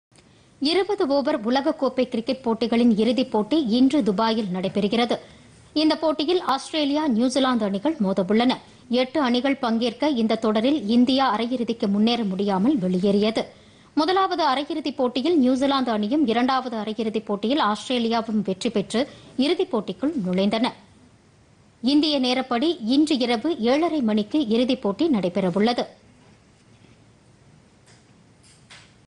டி-20 உலகக் கோப்பை கிரிக்கெட் இறுதிப் போட்டிPodhigaiTamilNews
20 ஓவர் உலகக் கோப்பை கிரிக்கெட் போட்டி - இறுதி ஆட்டத்தில் ஆஸ்திரேலியா - நியூசிலாந்து அணிகள் இன்று மோதல்